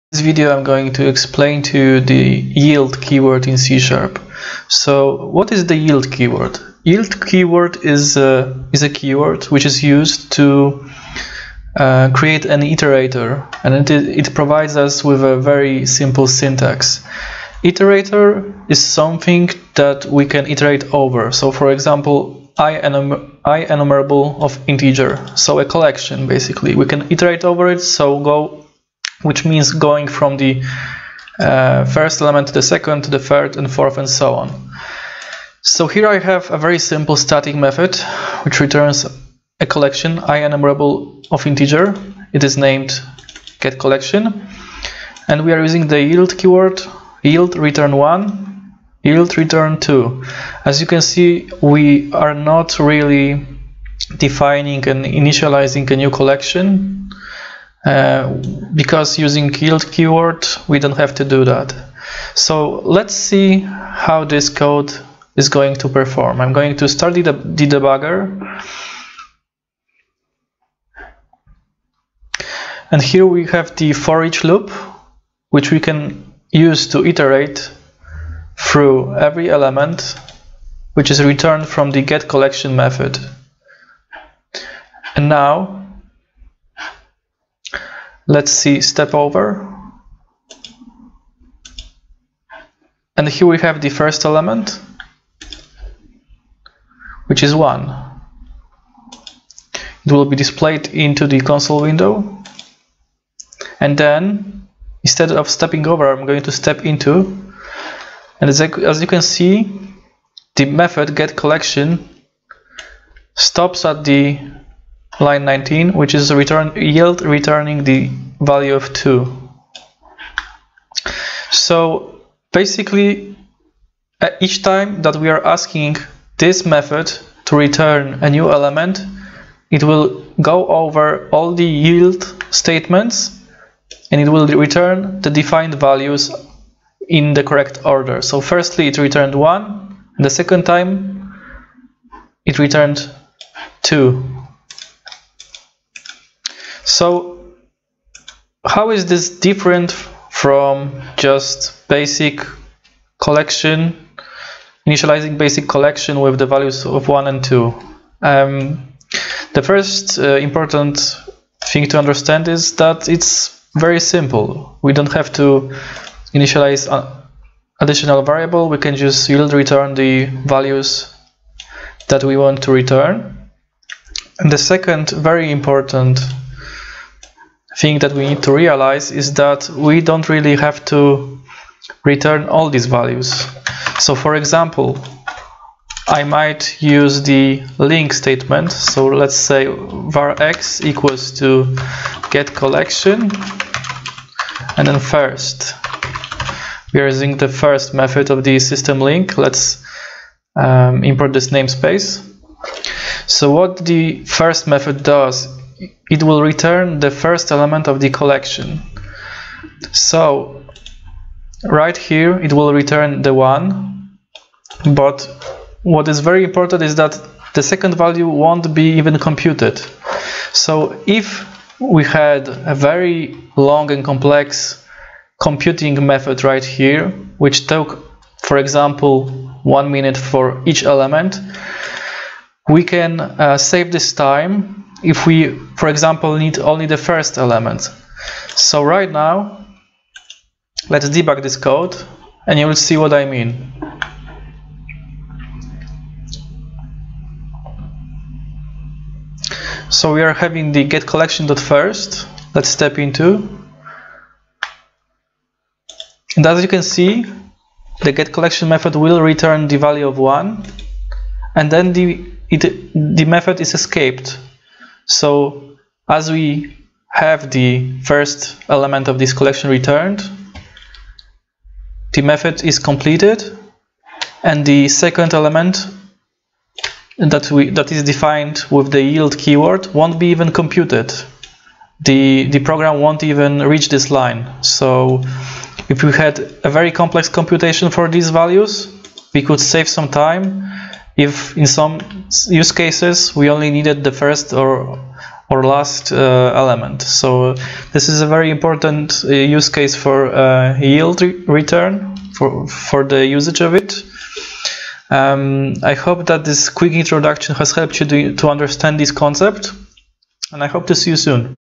Yield keyword in C#
In this video I explain the yield keyword in C#.